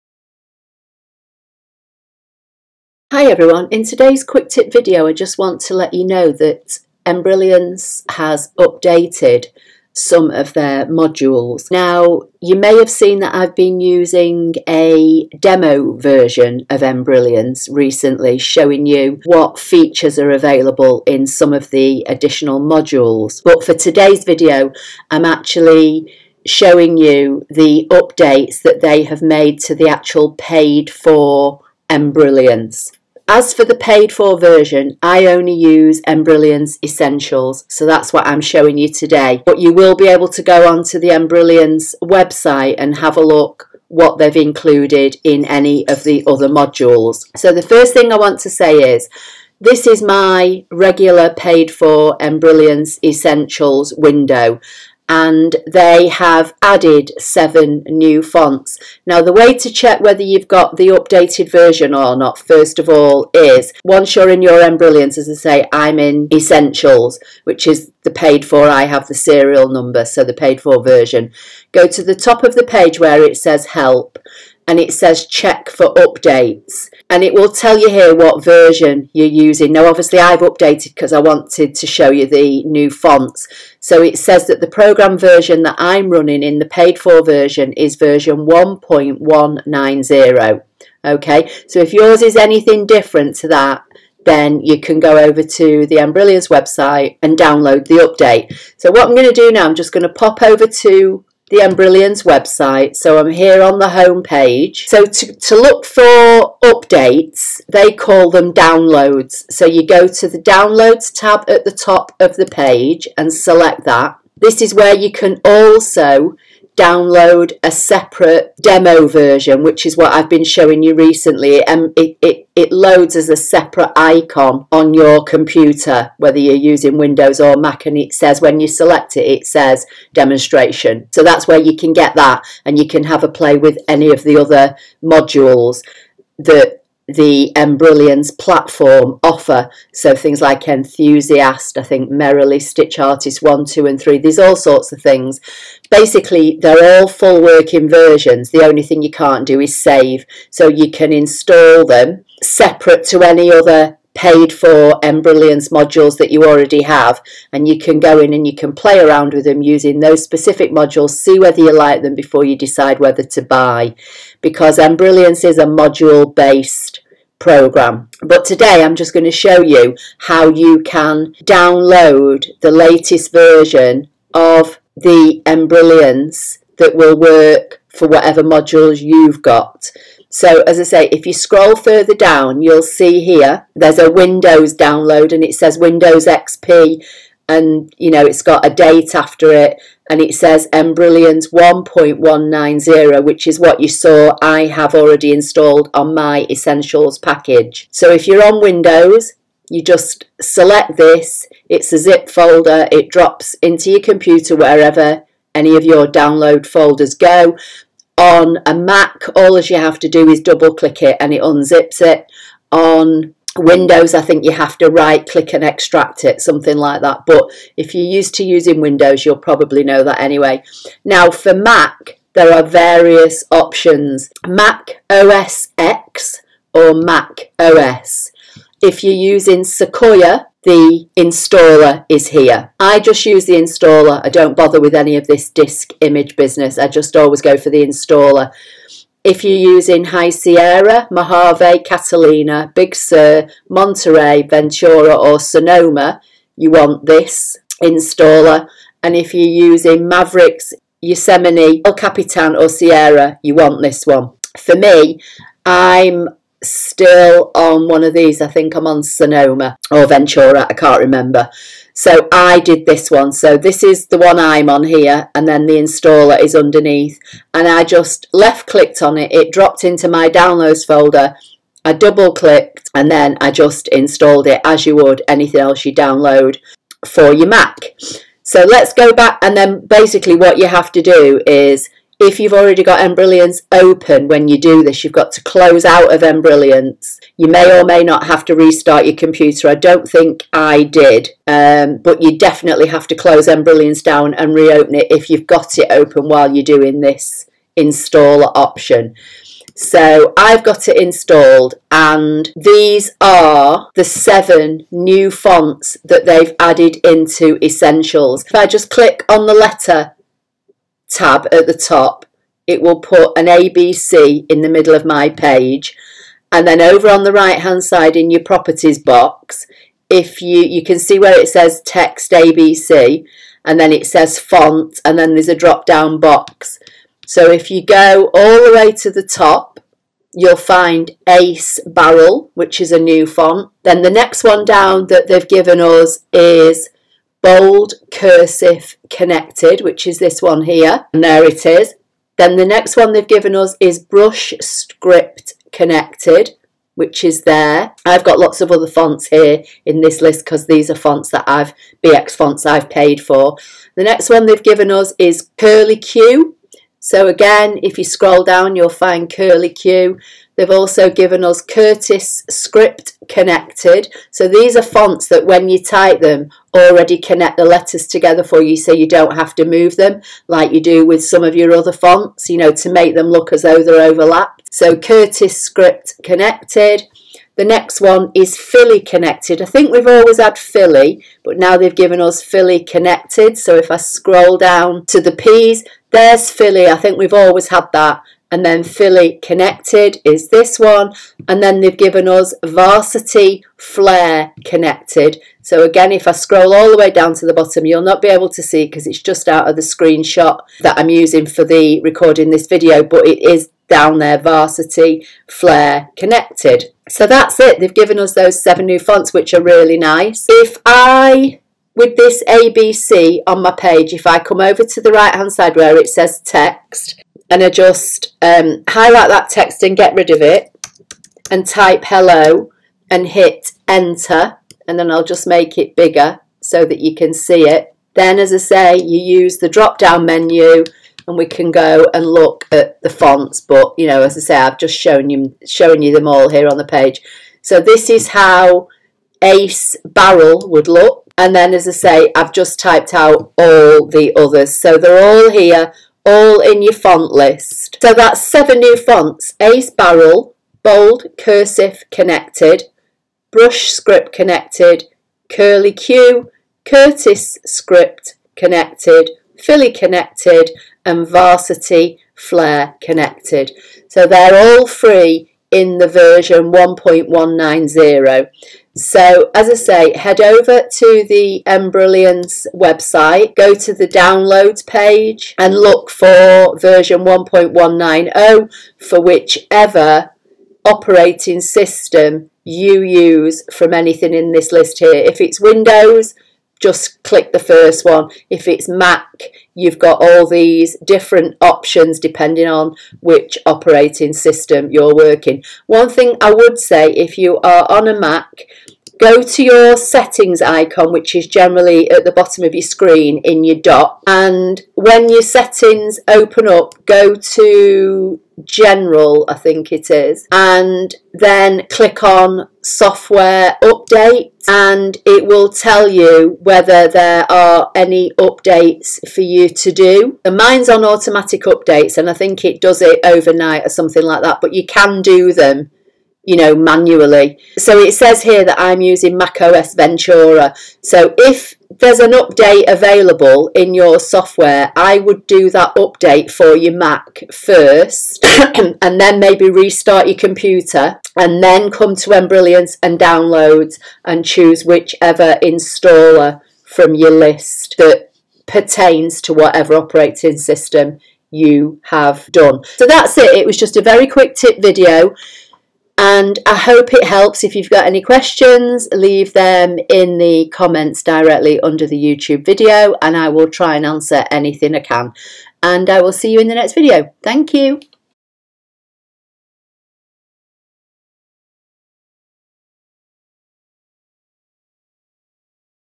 7 New Fonts In Embrilliance Essentials - Lets Have A Look
* Weekly videos for the ScanNCut and Sewing/Machine Embroidery projects * In todays video i am shwoing you how to check for updates in embrilliance essentials and showing you the 7 new free fonts they have given us to use.
* I have hundreds of videos here on my channel all in playlists -